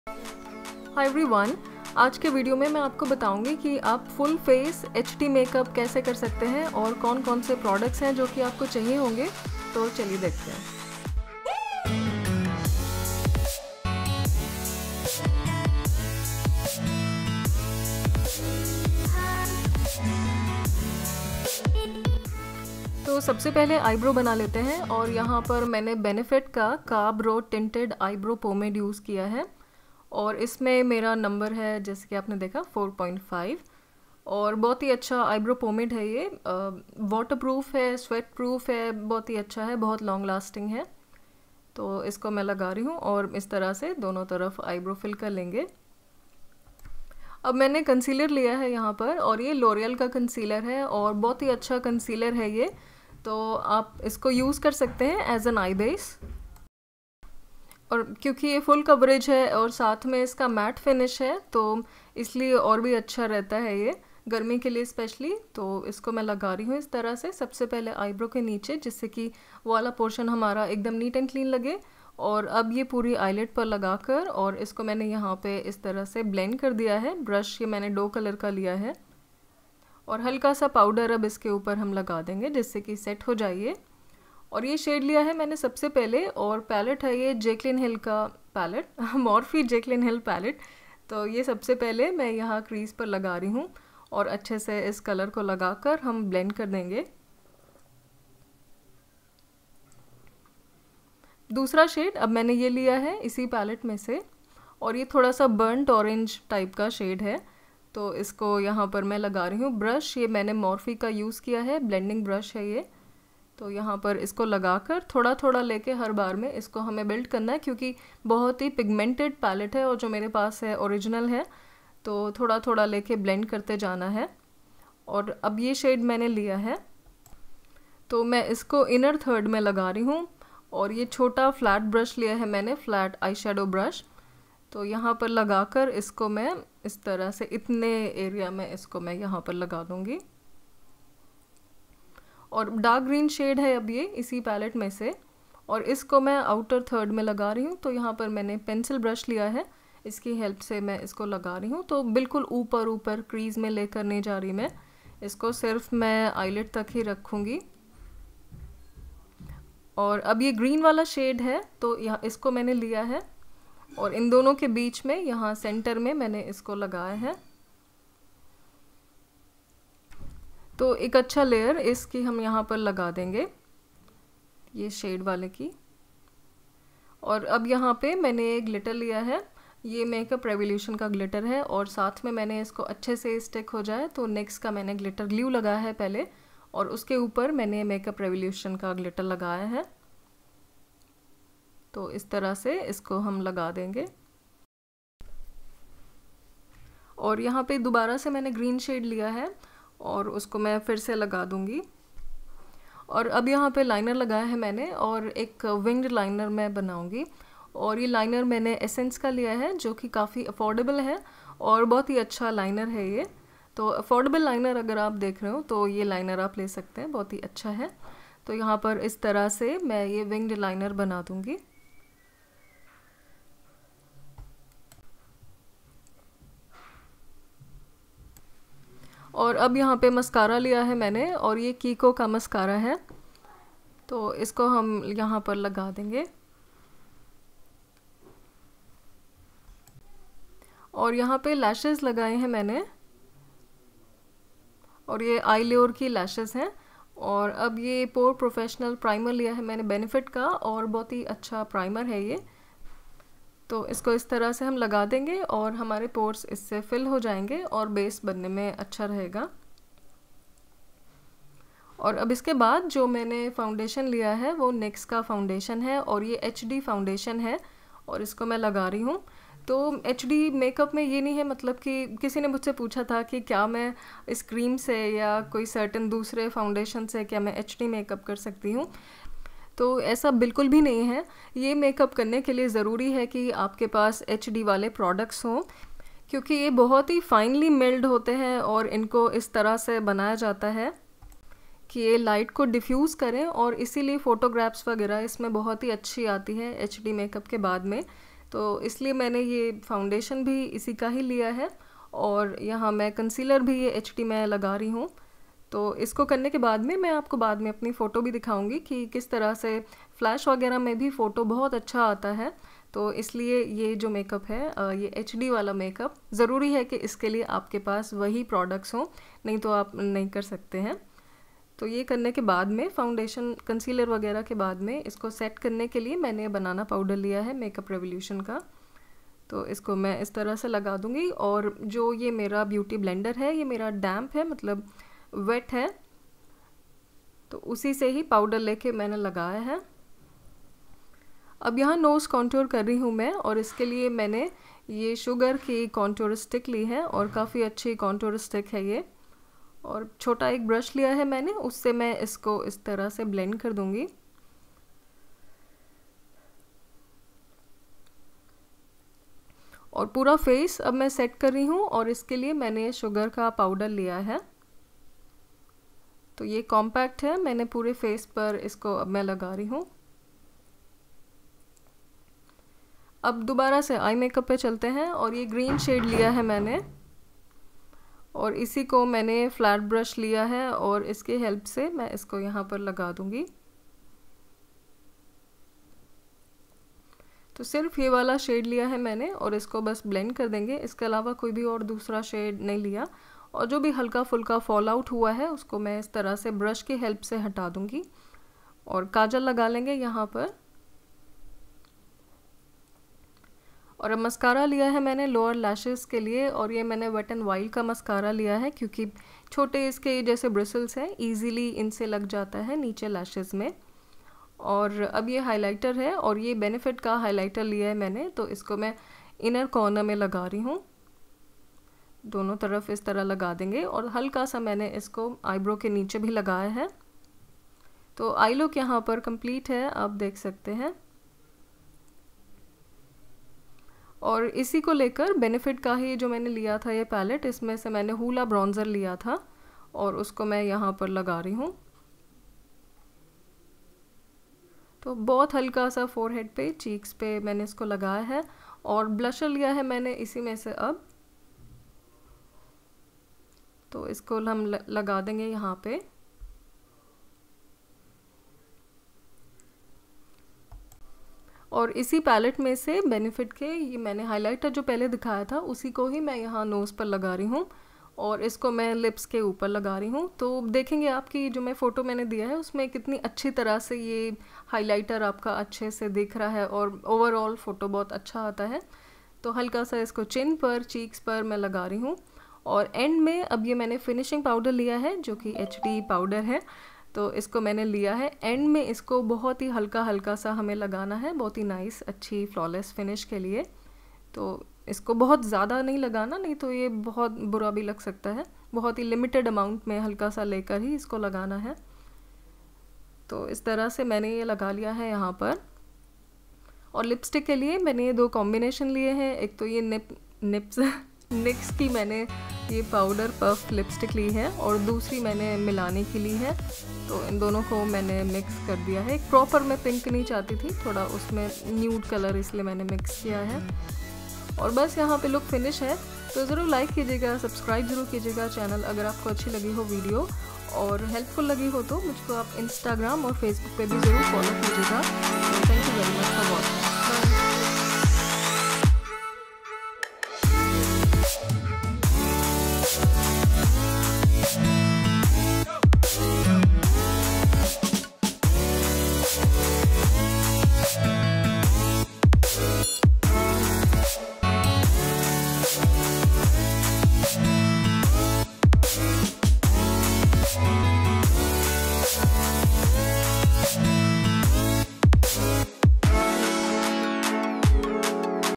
HD MAKEUP कैसे करें?क्या प्रोडक्ट्स चाहिए? FULL Face HD Indian Party Makeup Tutorial Smokey Eyes
Namaste in this video I will show you how to do HD Makeup and what products you will need to do it. I have done a deep teal glittery eye makeup and paired it with pink lipstick. Hope you guys like it :)

PRODUCTS USED:

Benefit Tinted eyebrow pomade 4.5
Loreal More Than Concealer 328 Biscuit
Morphe Jaclyn Hill Eyeshadow palette
Nyx Glitter glue
Makeup revolution Pressed glitter palette Abra Ka Dabra
Elf liquid ink eyeliner Black
Kiko Milano Unforgettable mascara
Benefit Pore Professional primer
Nyx HD Studio photogenic foundation 104 Sand Beige
Nyx HD Studio photogenic concealer CW 05
Makeup revolution loose banana powder
Sugar contour stick Fawn First 01
Sugar compact Coccocino
Benefit eye pencil black Bad Girl Bang!
Benefit Blush, Highlight and Bronzer palette Cheekleaders Pink Squad
Milani amore matte lip creme Loved
Nyx Powder puff lipstick Prank call

Eye lense: Air optix Sterling grey color